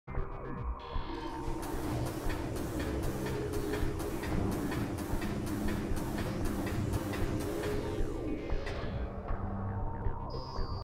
Pulse Dashing with Inputs
Video on Pulse Dashing originally by Hazel. Just uploaded with inputs for console use.